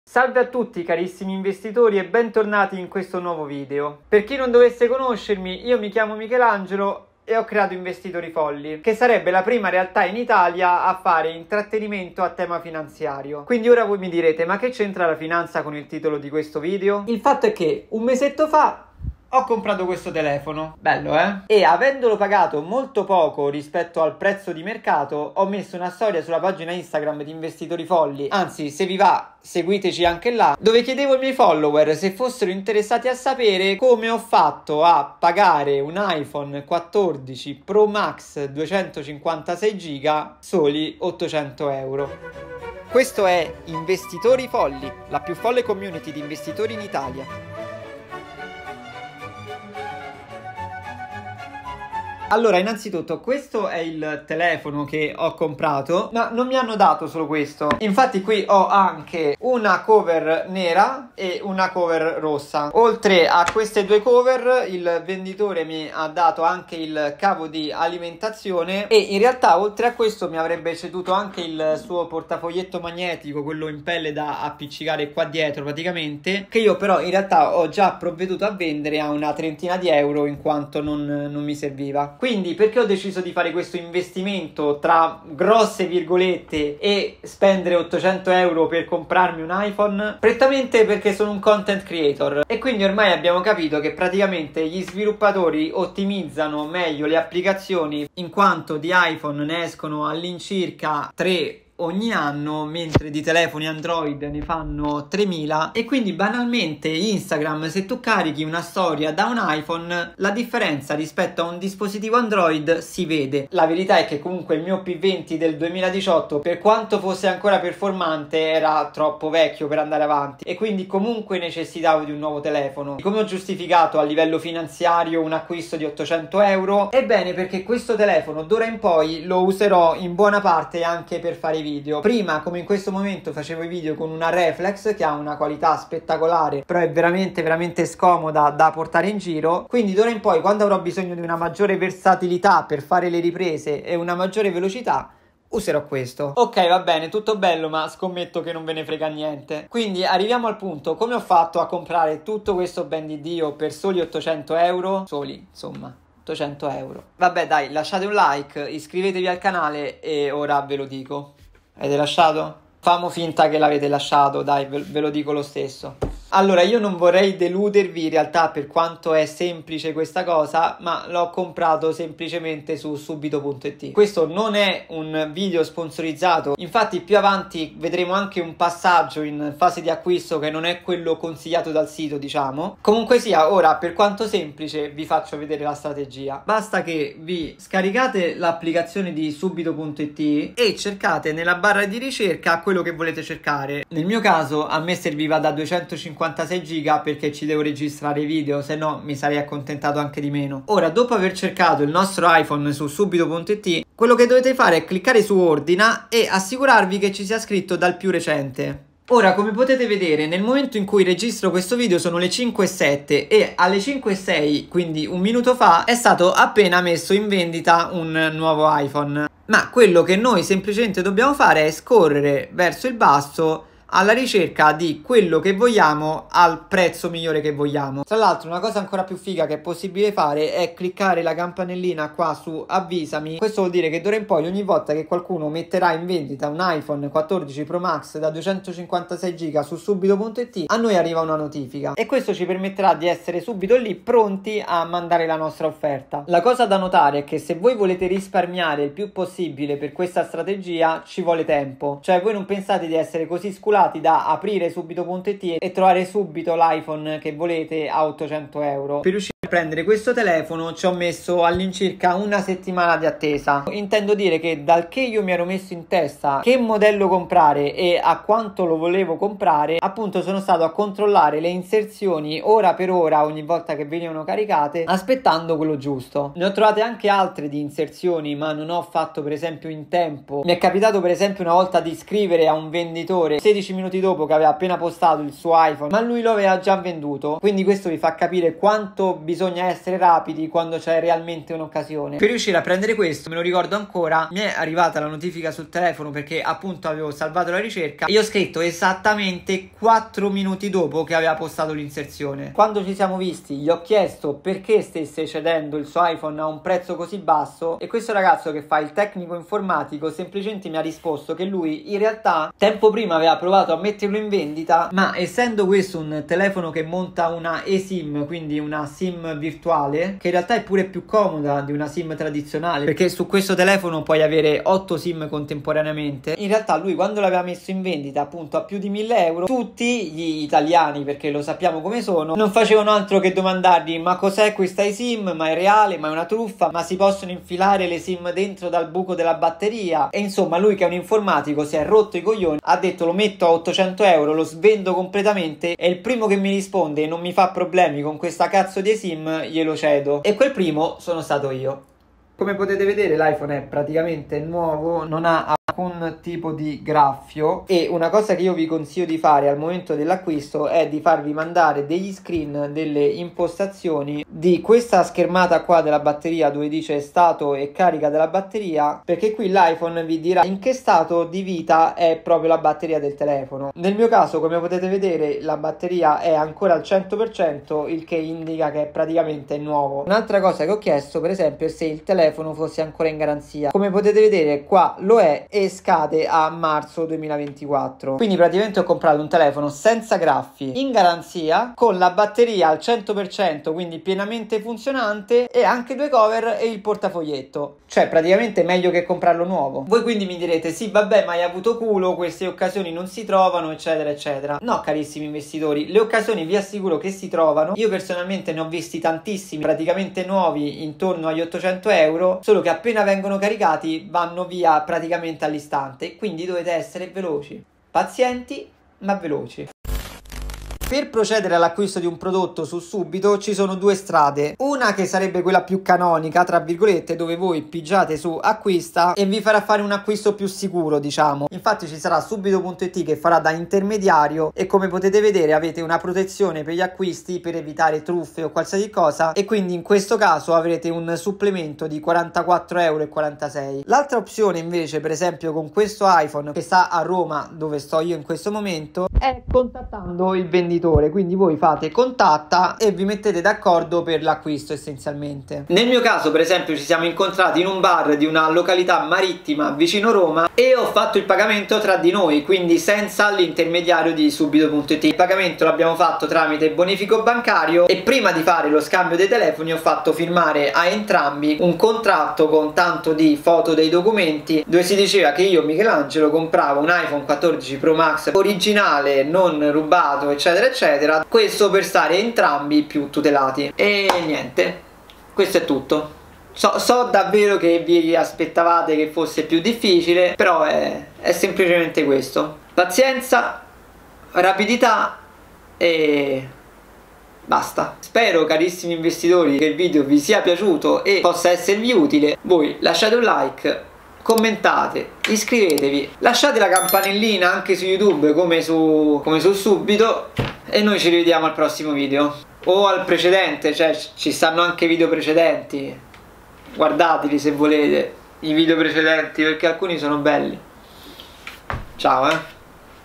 COME ho COMPRATO iPhone 14 PRO MAX 256gb a SOLI 800€ e perché
A grande richiesta dalla community di Investitori Folli vi spiego come e perché ho comprato un iPhone 14 pro max 256gb a soli 800 euro 💛

*MIGLIORI BROKER*
TRADE REPUBLIC (FINO A 200€ GRATIS)